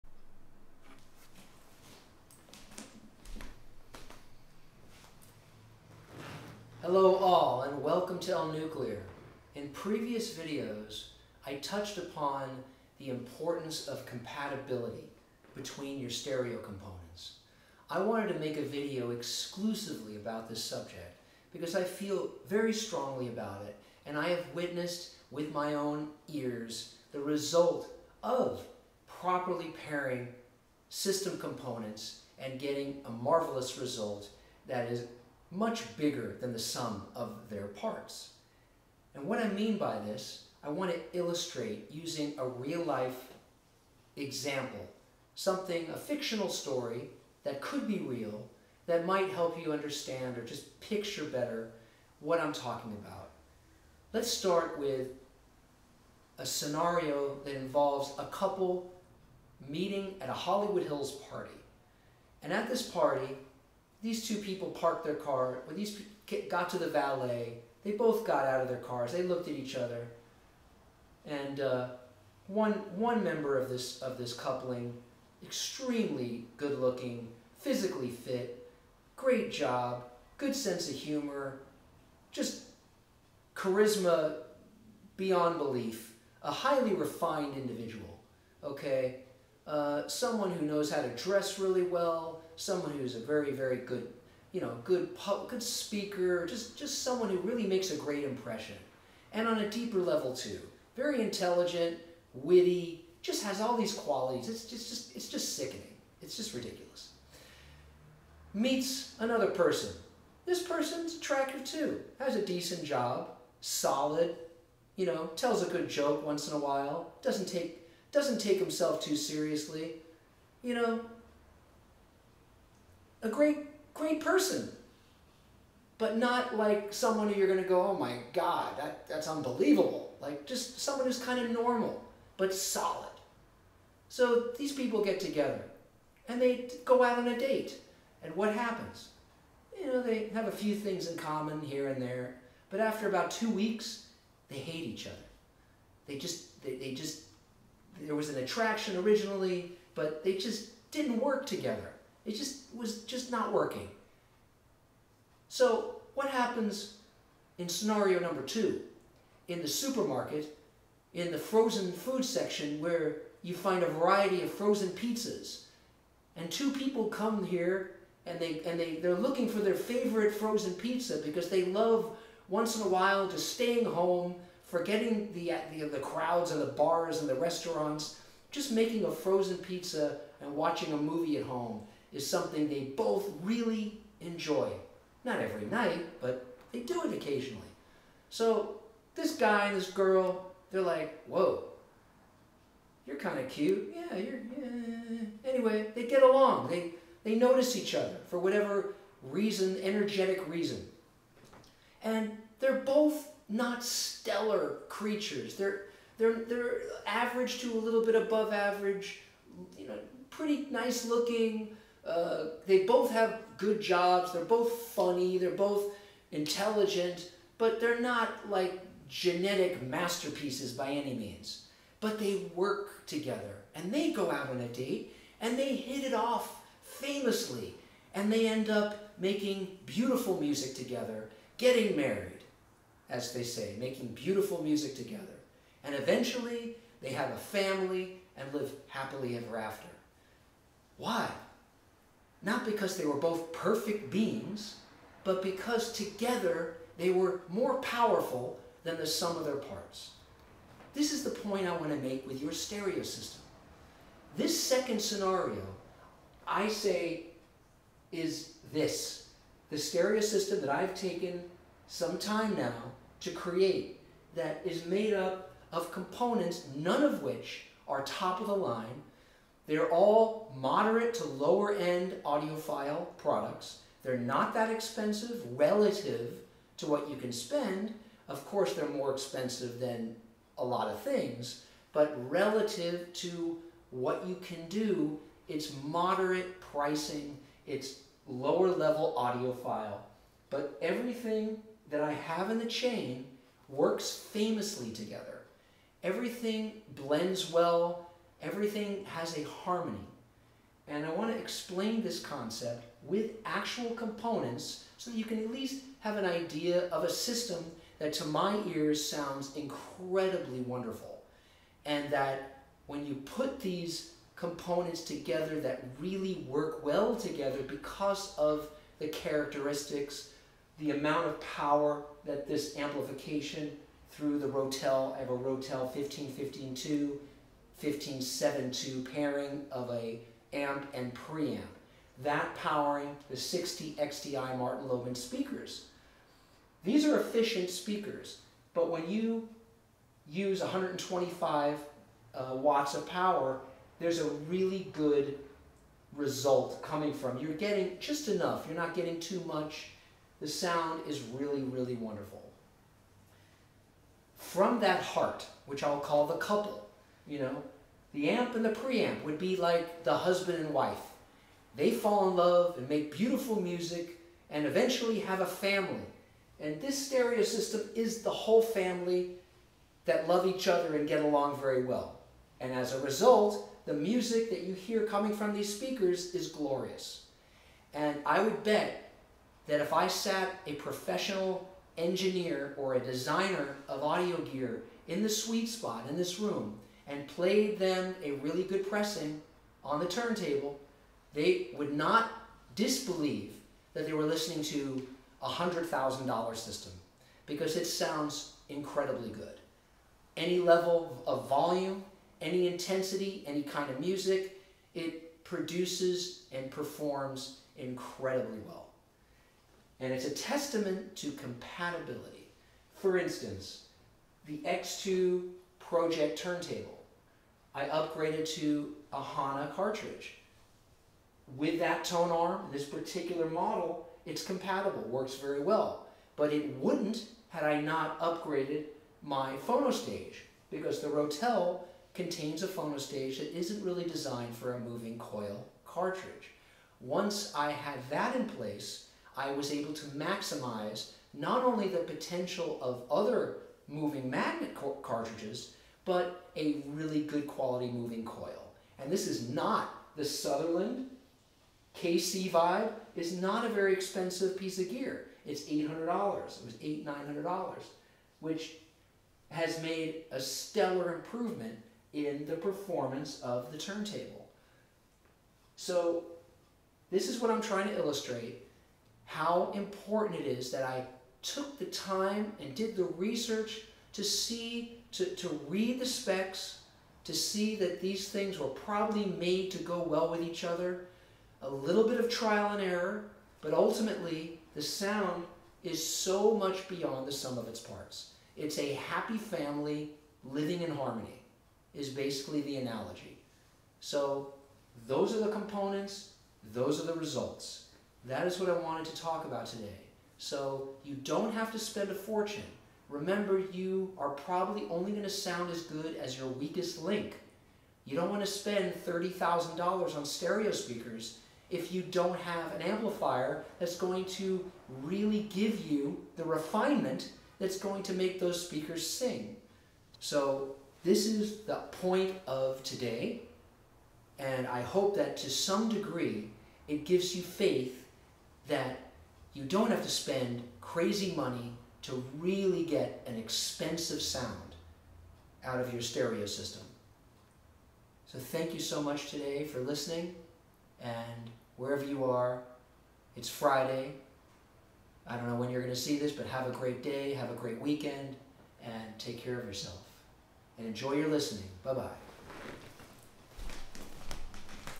Martin Logan speakers, Rotel power, Sutherland phonostage and Project X2. One big happy family?
Compatibility explored. Its importance for your stereo sound over expensive components.